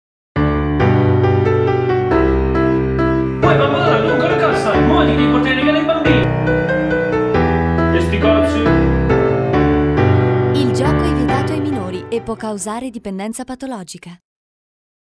Babbo natale gioca online
E' la vigilia di natale e babbo natale deve portare i regali ai bambini, ma con paddy power ha diritto di giocare pure lui.
Prova gratuitamente il casinò di Paddy Power -- Clicca qui per ricevere gratuitamente €20 di fun bonus e iniziare a giocare subito.